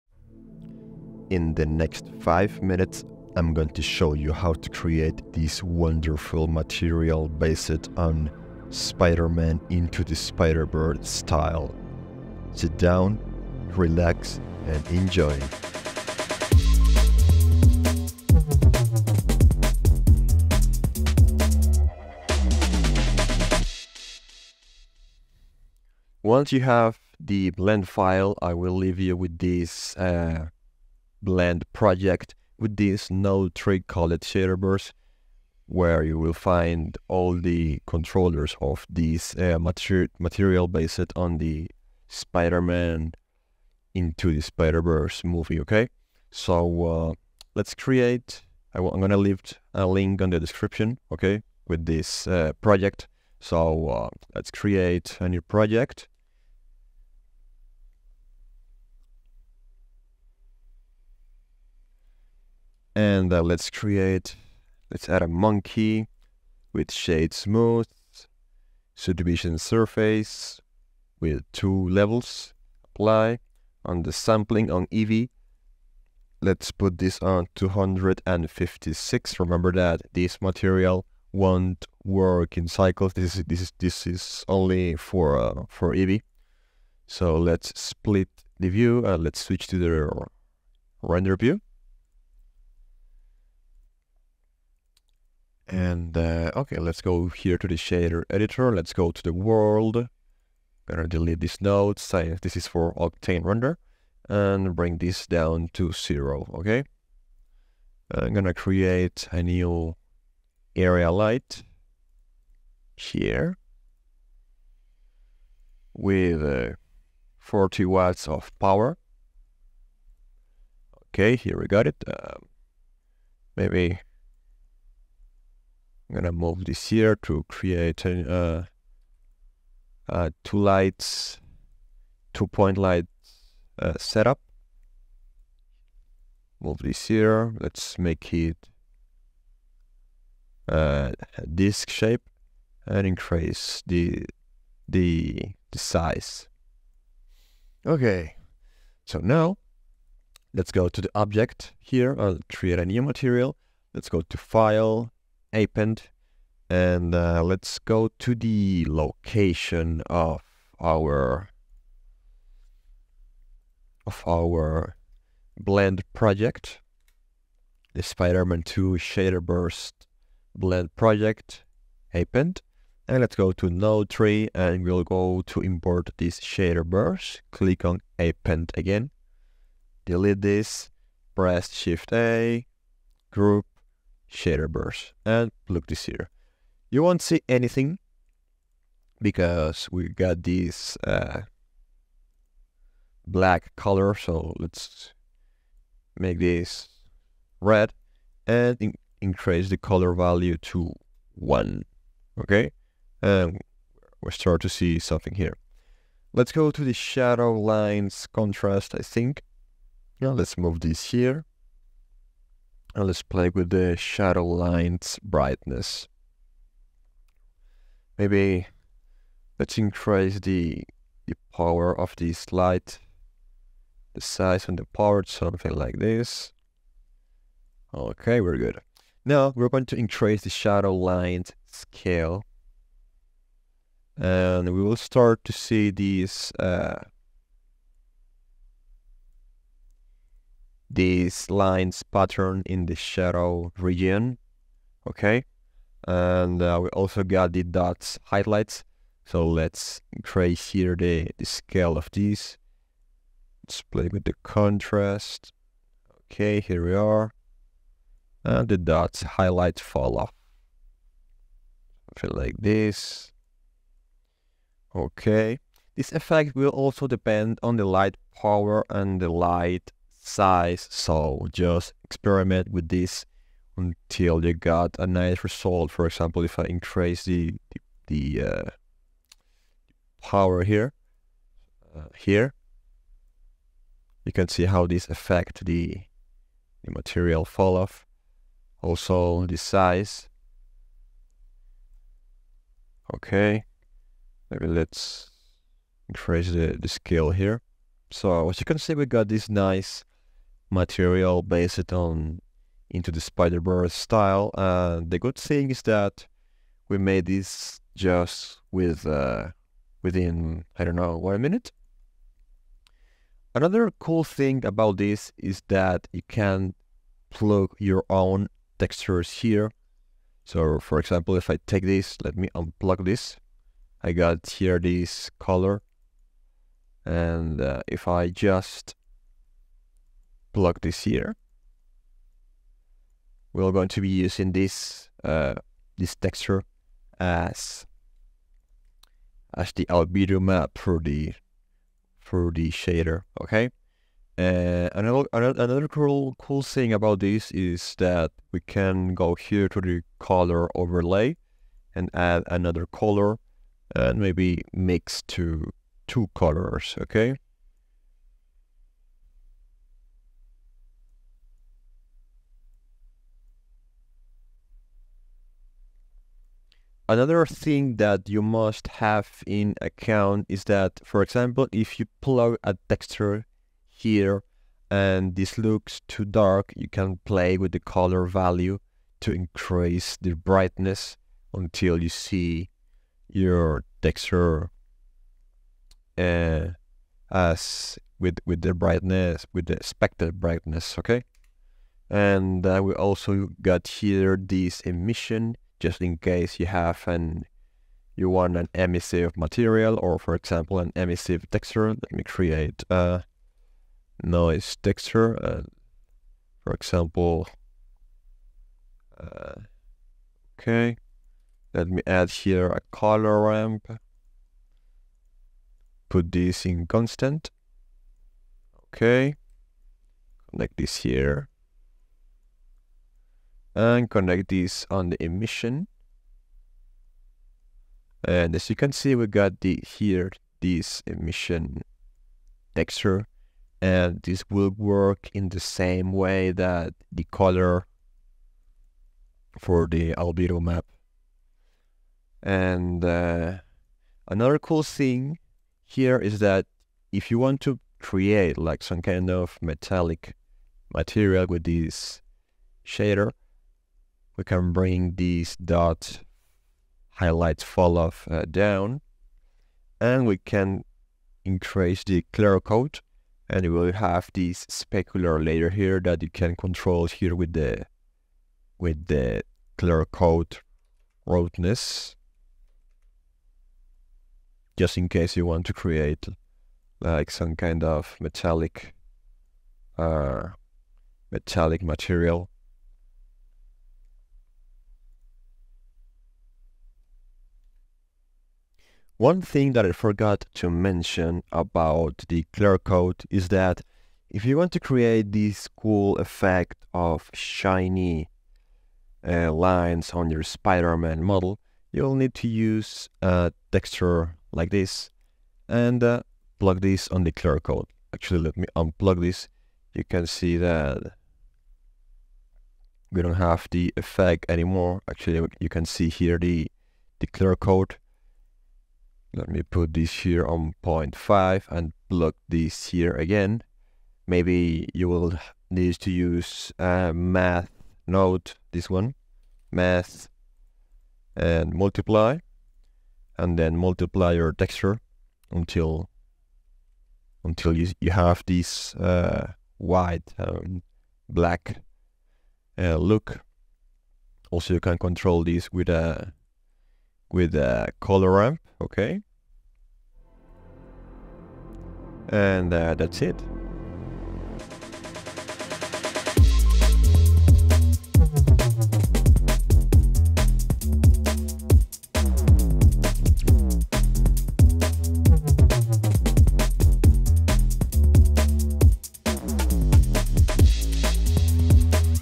Learn how to MASTER the Into The SPIDER-VERSE art style in Blender 🕷️
In this tutorial am going to show you how to create this shader based on Spider-man Into The Spiderverse style.